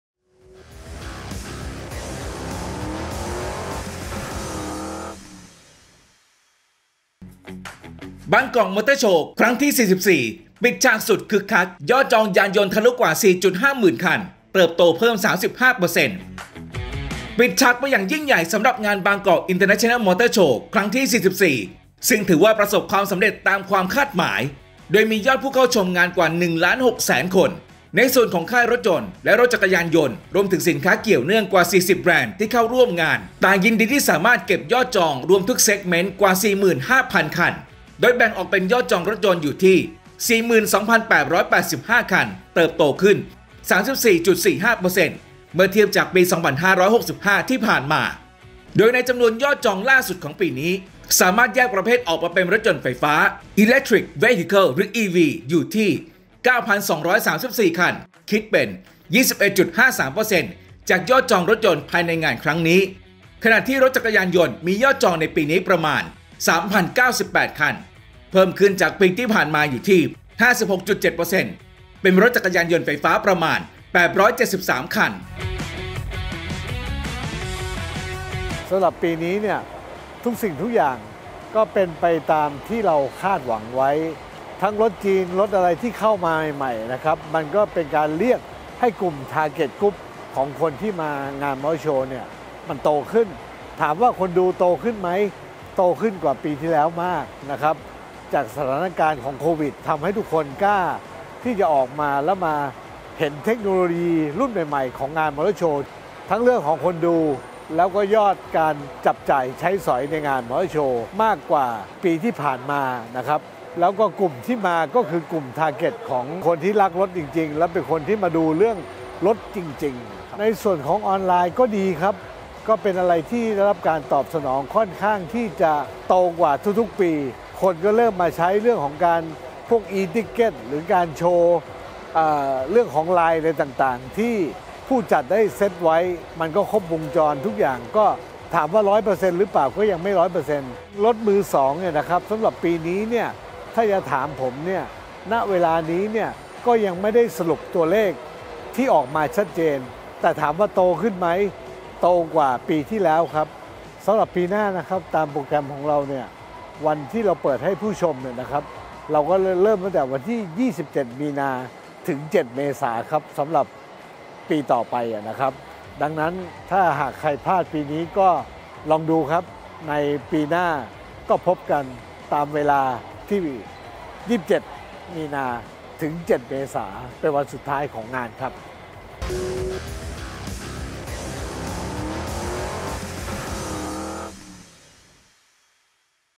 Motor Update | “บางกอก มอเตอร์โชว์” ครั้งที่ 44 ปิดฉากสุดคึกคัก ยอดจองยานยนต์ทะลุกว่า 4.5 หมื่นคัน
“บางกอก มอเตอร์โชว์” ครั้งที่ 44 ปิดฉากสุดคึกคัก ยอดจองยานยนต์ทะลุกว่า 4.5 หมื่นคัน เติบโตเพิ่ม 35%